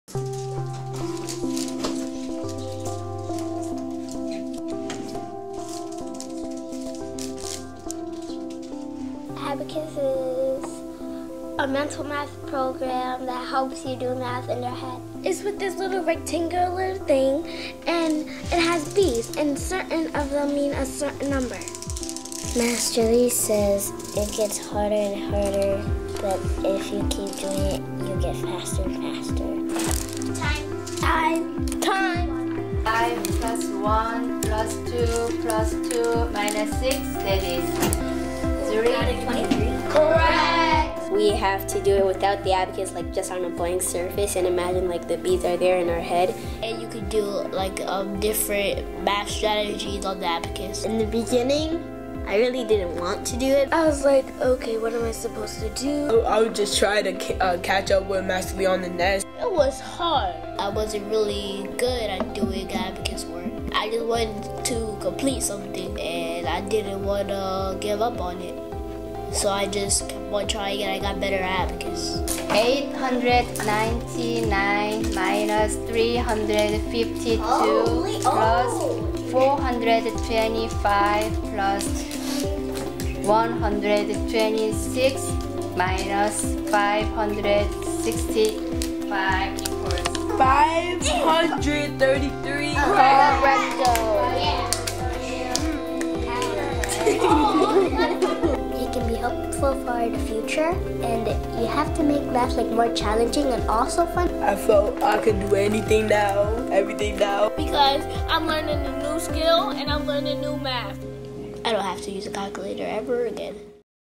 Grieco Elementary 80 Day Challenge
Third graders at Grieco Elementary worked on their abacus skills for 80 days, drastically improving their math skills, concentration, and most importantly their confidence in learning!